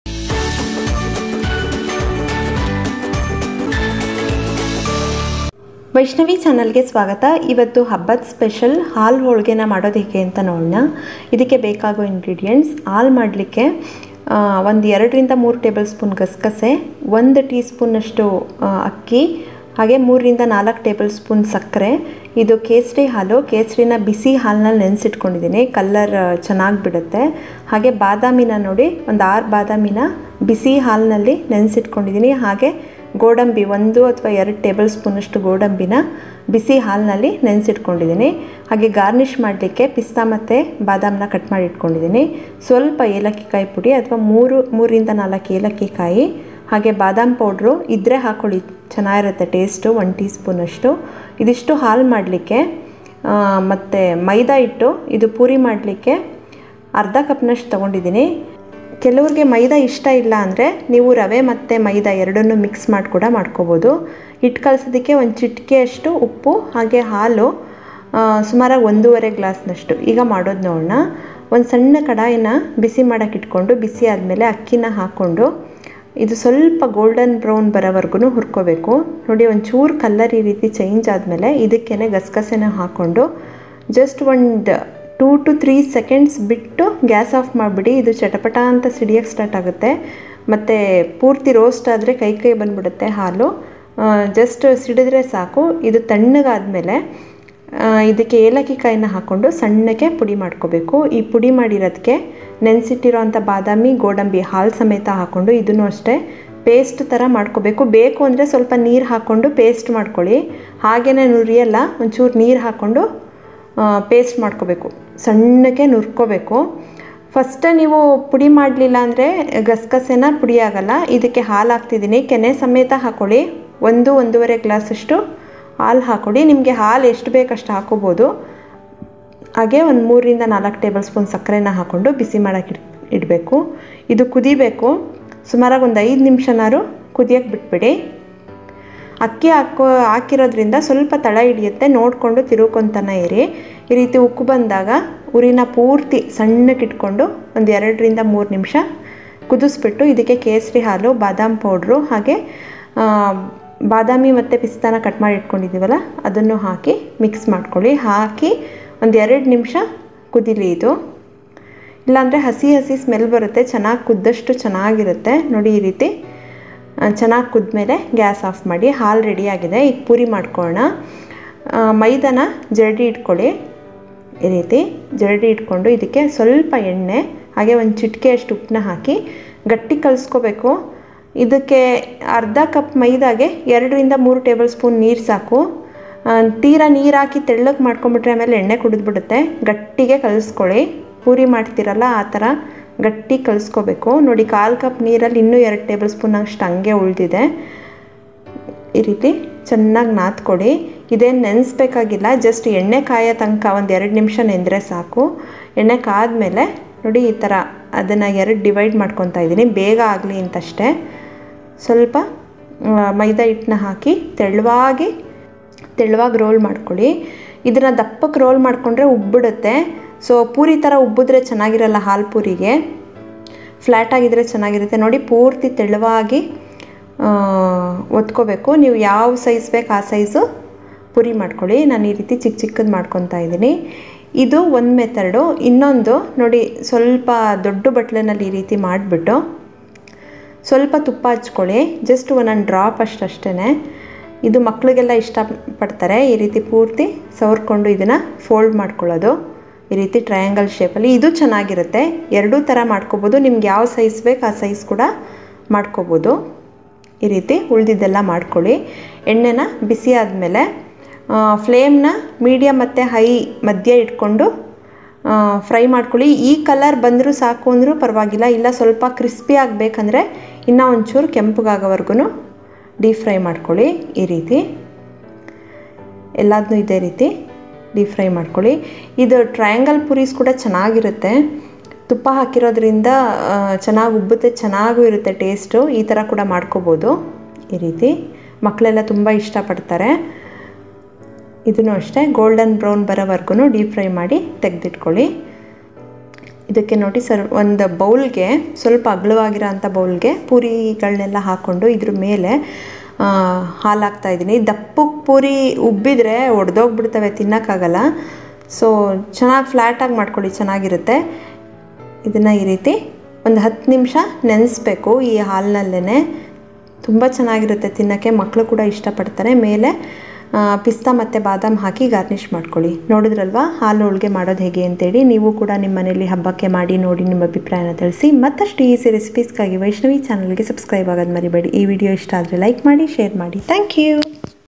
Haalu Holige / ಹಾಲು ಹೋಳಿಗೆ / Poori Payasa / Haalu Obbattu / Karnataka Festival Recipes
Karnataka Special Festival Recipe - Haalu Holige. too easy to prepare. Haalu Holige is a traditional Karnataka sweet dish. The crispy fried pooris are dunked in a mildly sweetened milk and can be served hot or chilled.